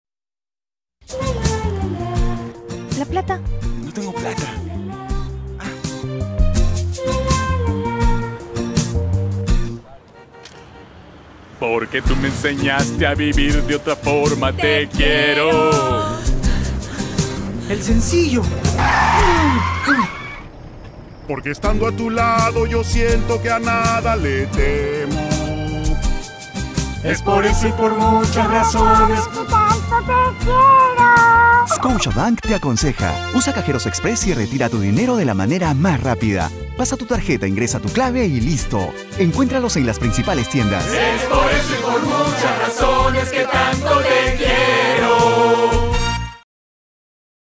Scotiabank Vrs Bebé
Comercial de Scotiabank cajeros express con una escena que no salió al aire.